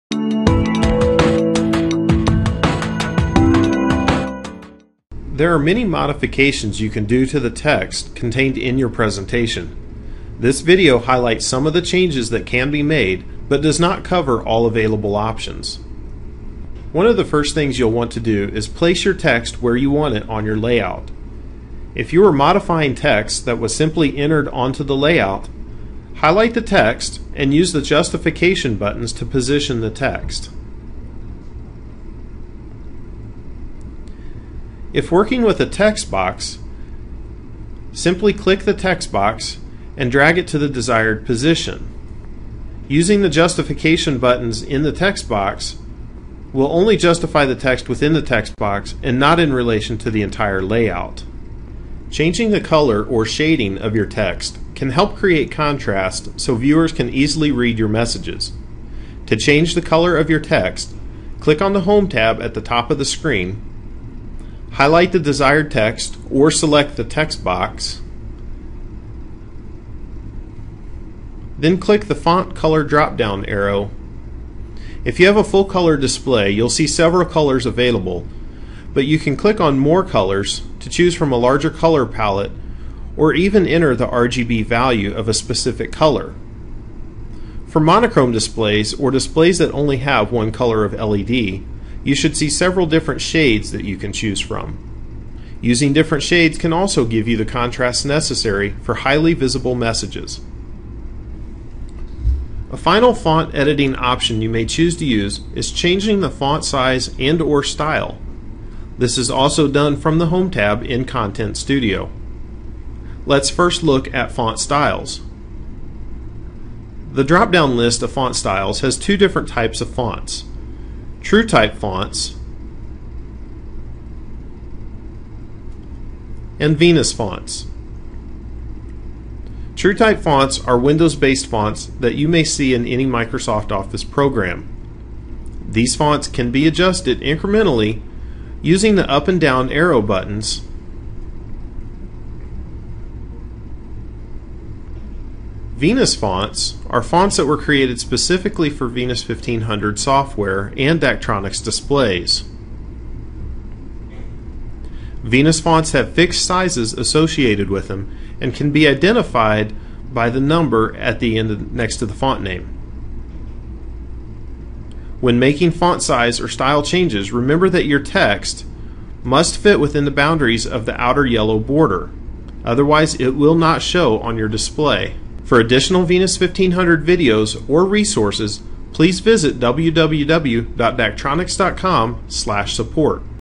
Venus 1500 Version 4: Modifying Text
How do I modify the text in my presentation?

This video explains some of the different text modifications you can do such as font style, font size and justification.  This video highlights only some of the modifications that can be done, but the process is similar for the other available modifications.  for more tutorials and information on Venus 1500 message display software.